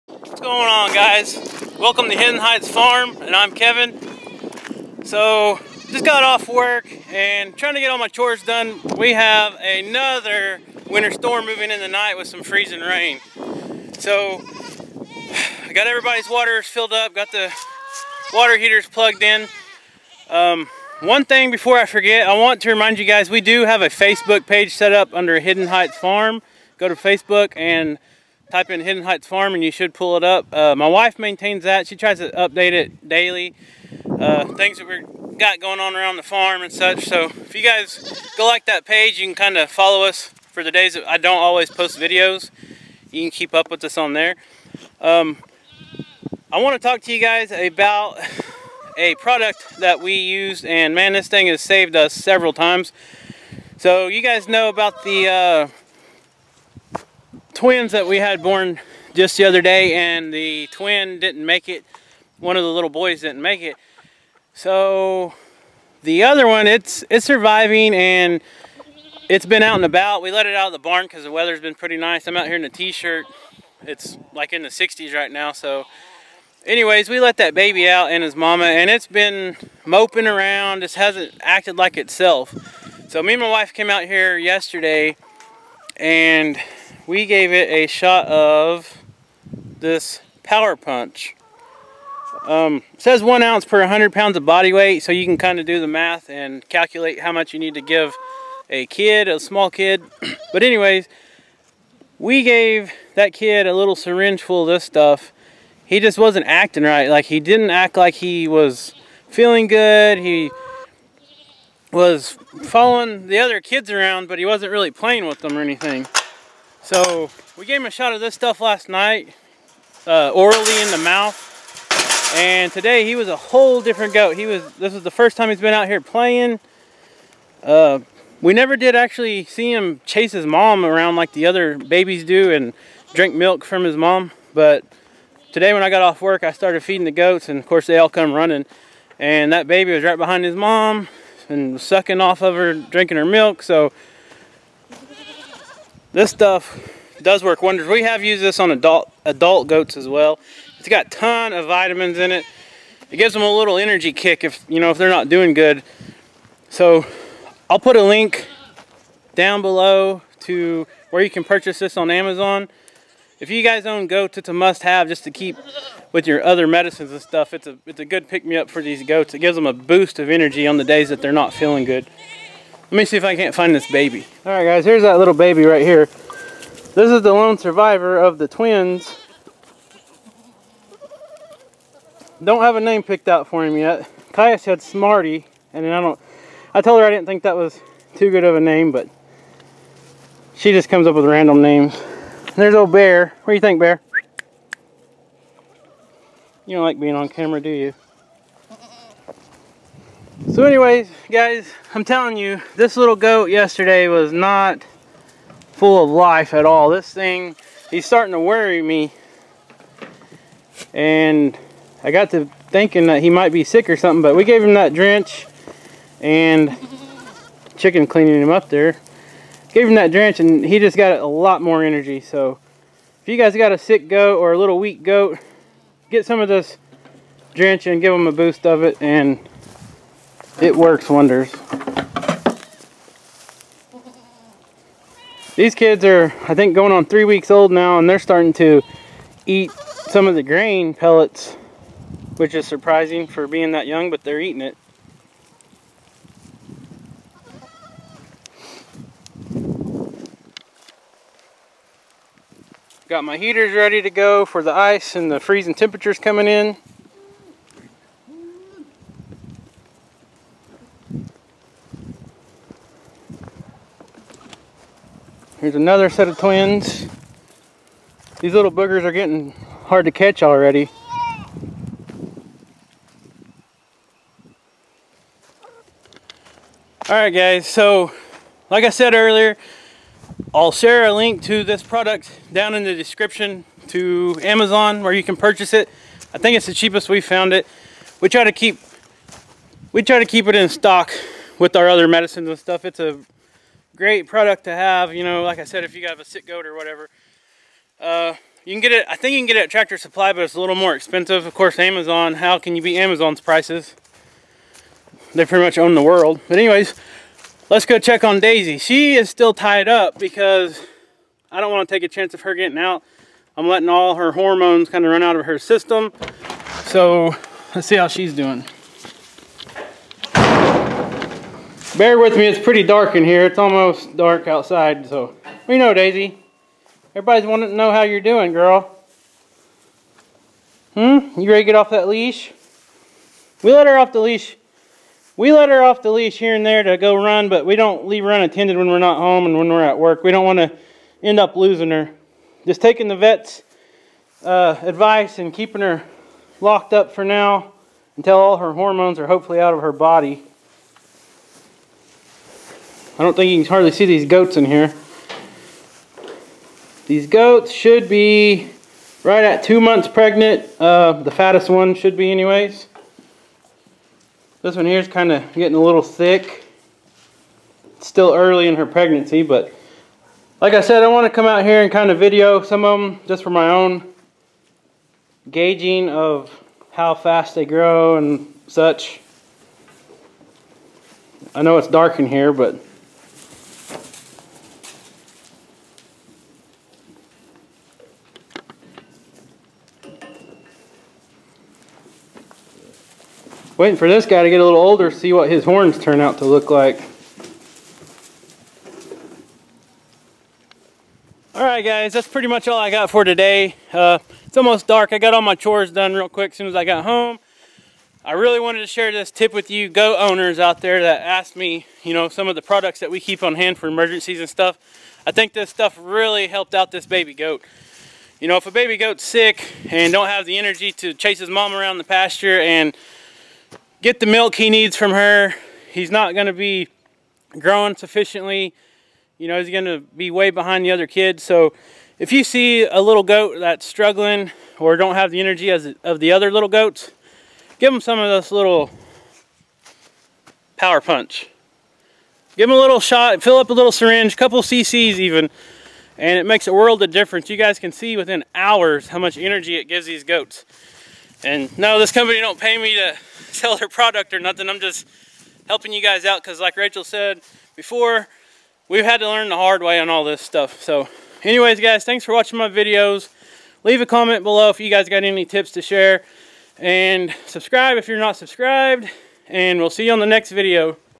How To Save A Weak Goats Life
We had a weak baby kid goat. We noticed the baby wasn't as active as the other goat kids. It wasn't following its momma around much or drinking enough milk. We gave the baby a few CC's of Power Punch in a syringe. The goat had instant energy back in a matter of hours. This goat is acting like a new goat already. He's been playing with the other kids and very active. Power Punch is a great drench for weak goats.

Link to buy Power Punch on Amazon

Hidden Heights Farm
Salina, Ok. 74365